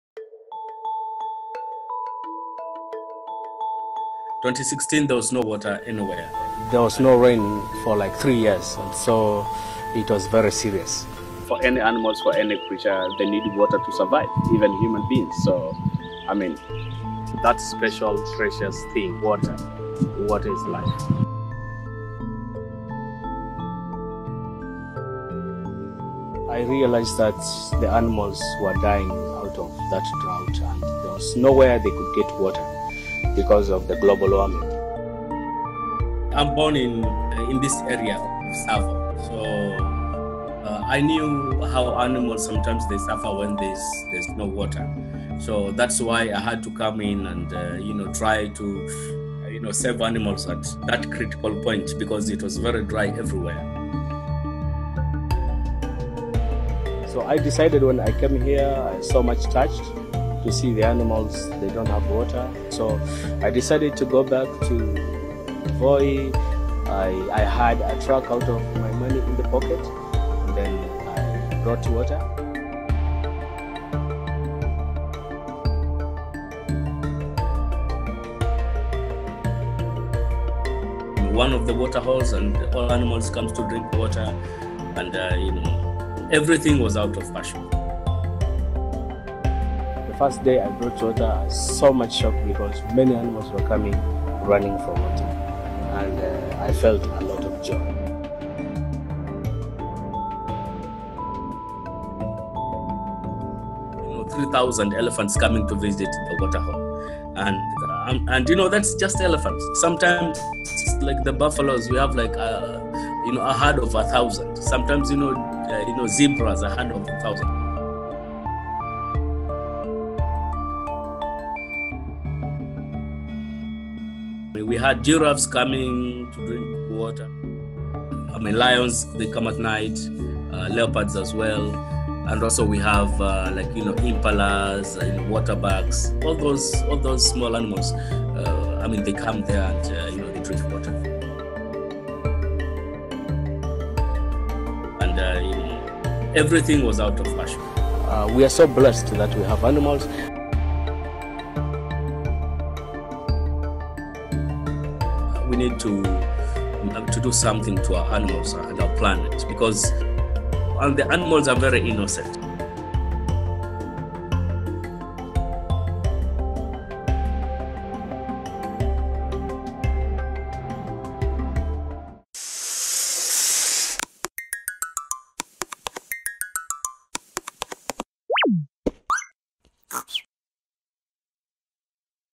Man Saves Wild Animals By Giving Them Water! | The Koala
Patrick delivers water to thirsty animals all over Kenya! ❤️